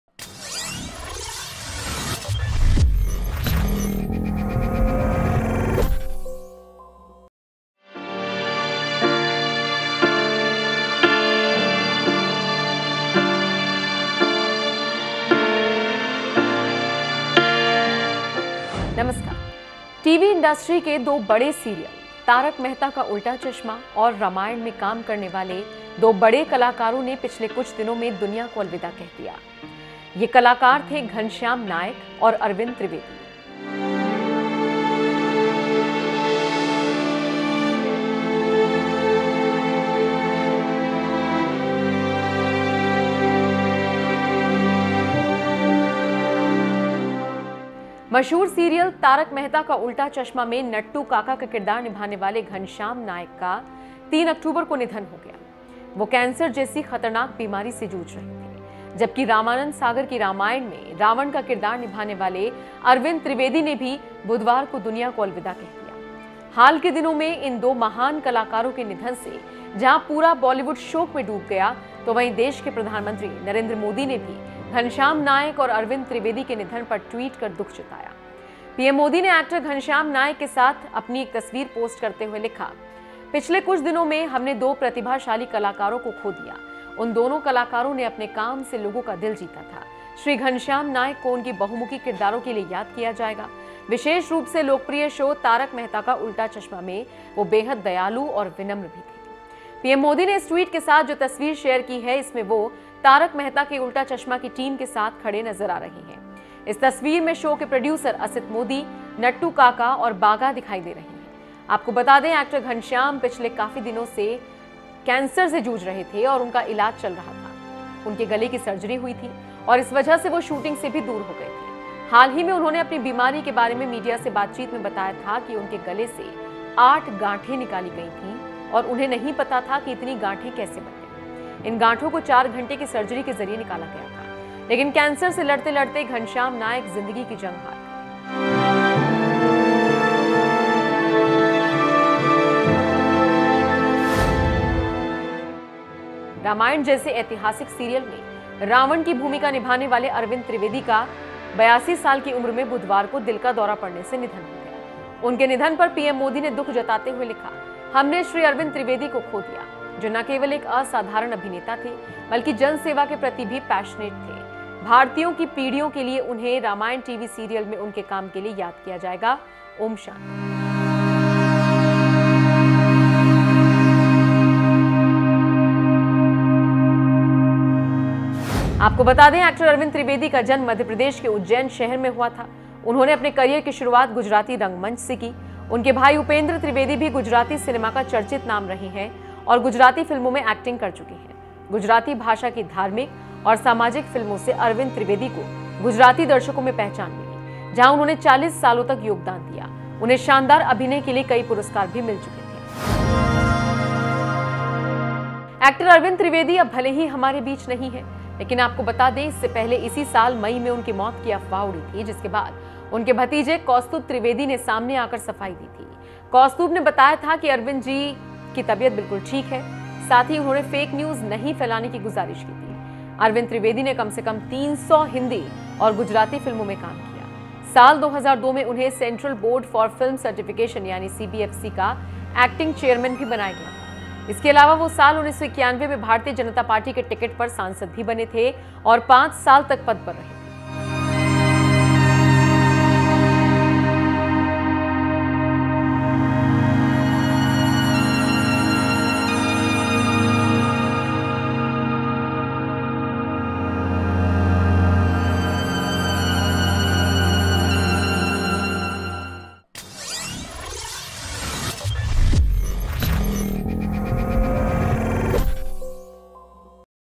Ghanshyam Nayak और Arvind Trivedi के जाने से टूट गये PM Modi !
Actor Ghanshyam Nayak और Arvind Trivedi के जाने से टूट गये PM Modi, देखिये तस्वीर शेयर कर क्या कहा !